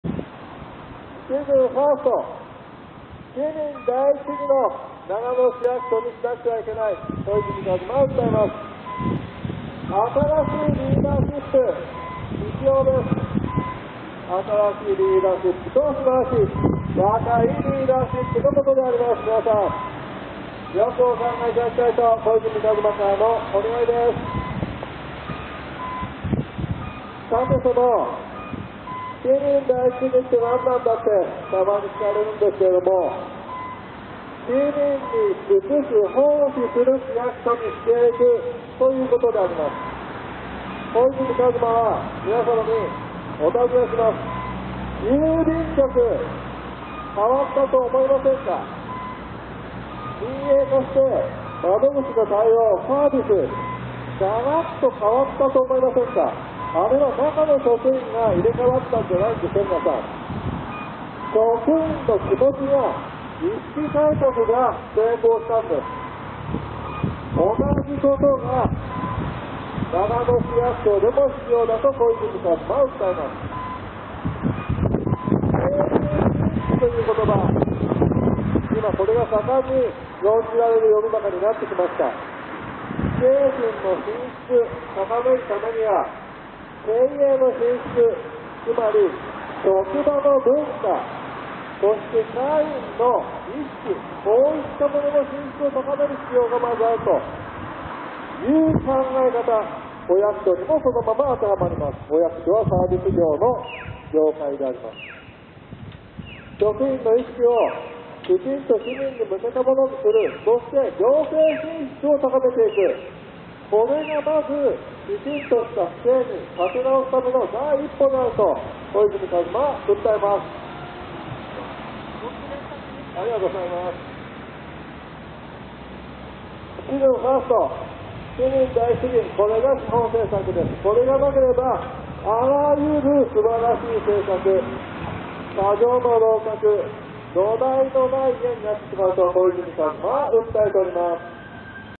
シチズン・ファースト-市民第一主義を訴える：新兵器(笑)登場! 2013.09.29
シチズンファースト-市民第一主義は、市民満足度の向上を重視する。
郵便局は、民営化してサービスの品質が上がったが、それがいちばん良くわかるのは窓口の応対。郵便局の職員が入れ替わったわけではなく、顧客満足度を上げていくことが重要だと、職員が認識した結果。製品・サービスの品質を上げるには、職場の文化や職員の意識改革が必要。
長野市でも同じことが言える。市民に奉仕する市役所との意識がなければ、あらゆる施策はやっつけ仕事となり、形だけのものになりかねない。市民第一主義は全ての土台であり、これなくして真の意味での行政改革はできない。
新兵器(笑)ののぼりとともに訴える。